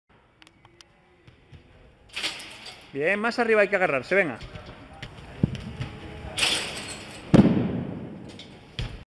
Polvoltim Drills. Batida a agarrarse muy alto en la cuerda.
Publicaciones sobre salto con pértiga y novelas de ficción - FACEBOOK del Polvoltim